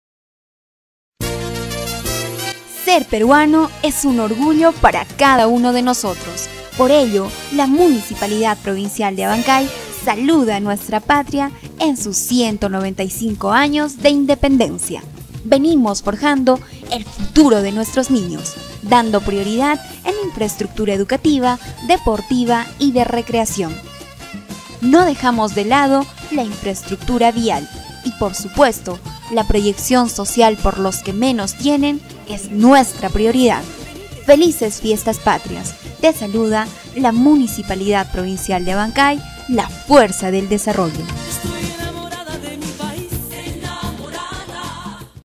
SALUDO POR FIESTAS PATRIAS
SER PERUANO ES UN ORGULLO PARA CADA UNO DE NOSOTROS, POR ELLO LA MUNICIPALIDAD PROVINCIAL DE ABANCAY
SALUDA A NUESTRA PATRIA EN SUS 195 AÑOS DE INDEPENDENCIA
VENIMOS FORJANDO EL FUTURO DE NUESTROS NIÑOS DANDO PRIORIDAD EN INFRAESTRUCTURA EDUCATIVA, DEPORTIVA Y DE RECREACIÓN
NO DEJAMOS DE LADO LA INFRAESTRUCTURA VIAL, Y POR SUPUESTO LA PROYECCIÓN SOCIAL, POR LOS QUE MENOS TIENEN , ES NUESTRA PRIORIDAD
FELICES FIESTAS PATRIAS
TE SALUDA
LA MUNICIPALIDAD PROVINCIAL DE ABANCAY.